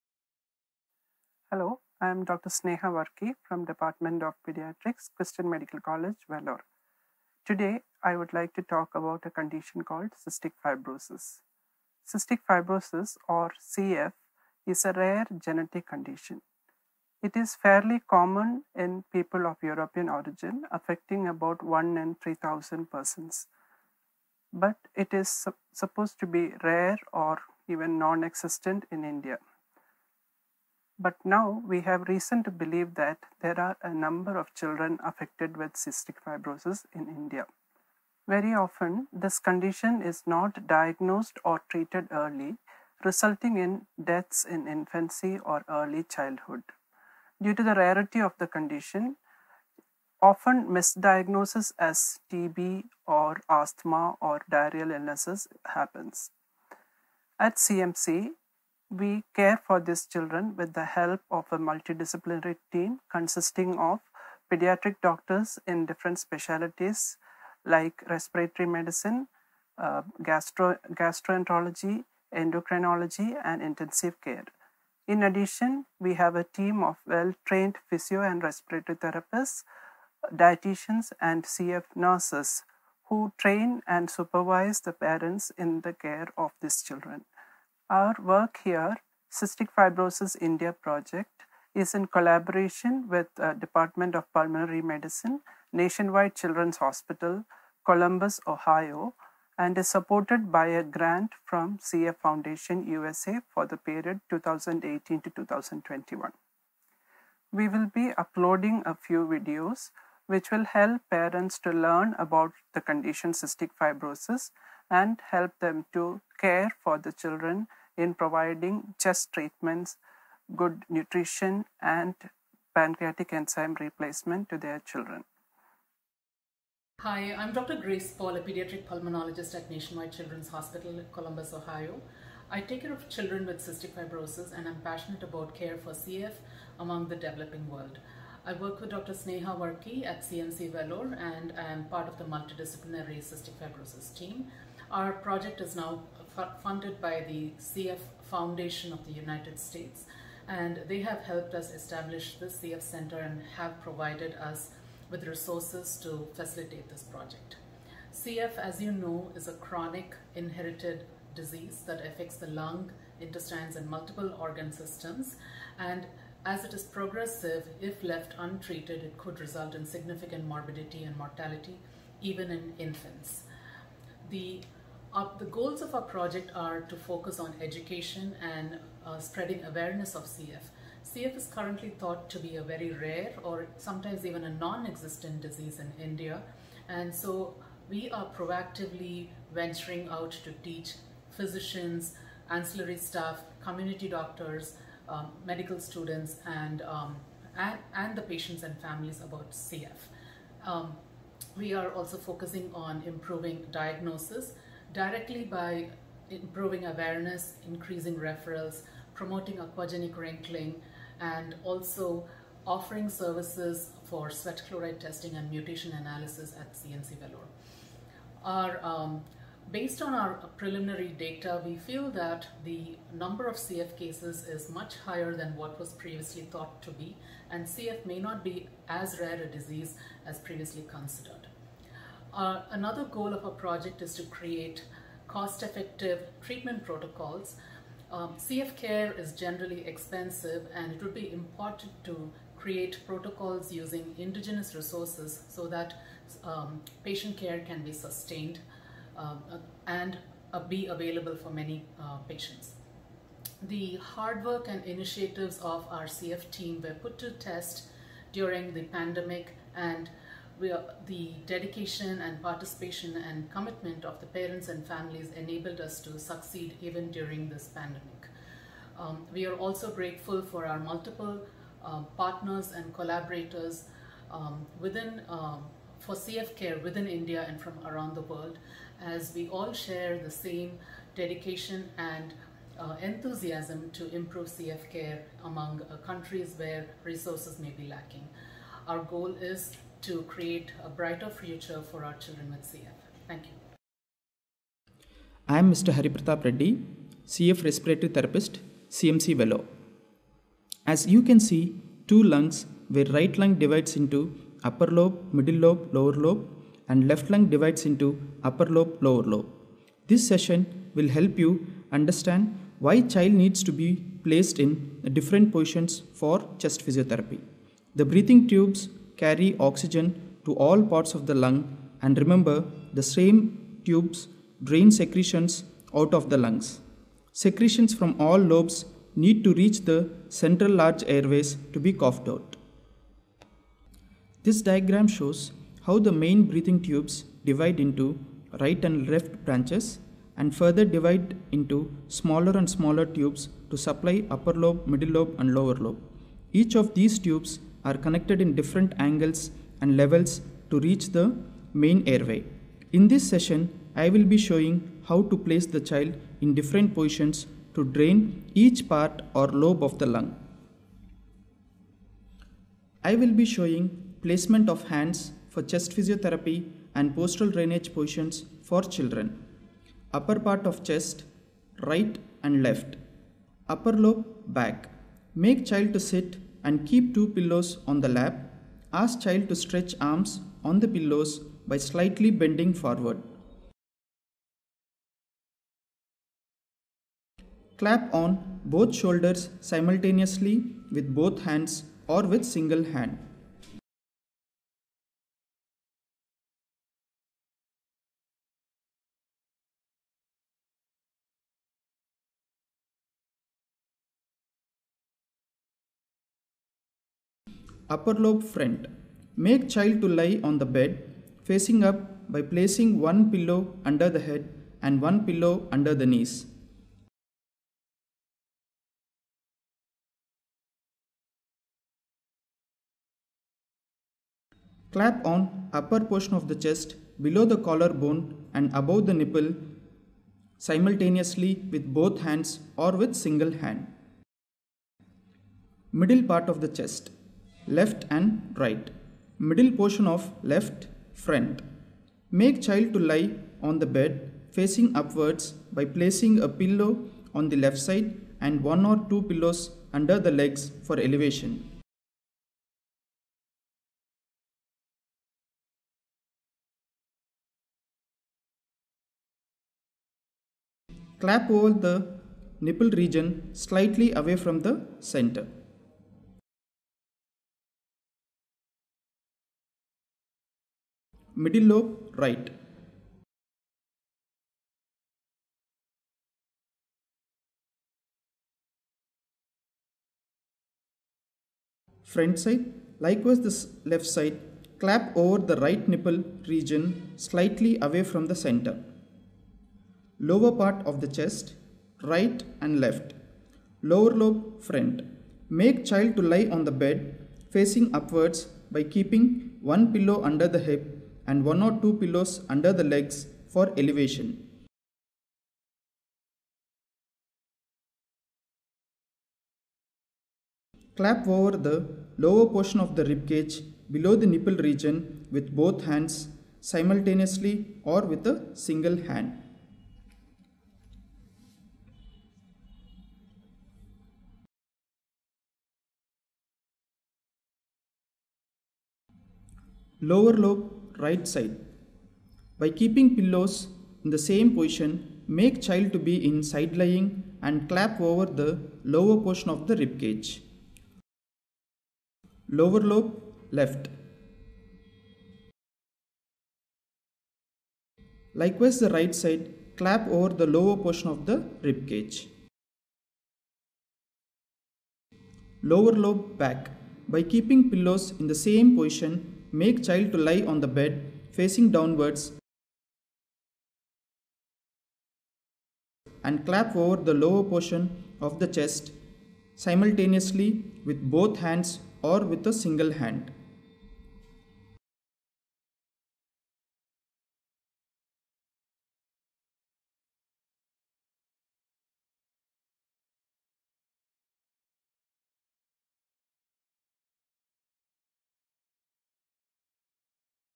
Chest Physiotherapy for Cystic Fibrosis
Christian Medical College Vellore provides holistic care for children with Cystic Fibrosis (CF), a genetic disease that affects multiple organs and can be fatal if left untreated.

A multidisciplinary CF care team was developed under the CF-India Project, a collaborative venture between the Department of Pediatrics (Respiratory services), CMC, Vellore and the Department of Pulmonary Medicine at Nationwide Children’s Hospital, Columbus, Ohio, USA , supported by the US Cystic Fibrosis Foundation.

The aims of the CF-India project are to create awareness about CF among Indian health care professionals and patients, facilitate early diagnosis, and help initiate basic CF-specific treatment to reduce morbidity and mortality.

Our CF care team is excited to share some educational videos created for parents to provide the best CF-specific care for our patients using resources that are generally available in India. Periodic check-ups with the medical team remain important to reinforce these treatments and address questions and updates.

In this video , Mr. Hari Prathap Reddy, paediatric respiratory therapist explains and demonstrates how chest physiotherapy should be administered to children.

Dr. Sneha Varkki, Department of Paediatrics,Christian Medical College, Vellore
Dr. Grace Paul, Paediatric Pulmonary and Sleep Medicine
Nationwide Children’s Hospital, Columbus, Ohio, USA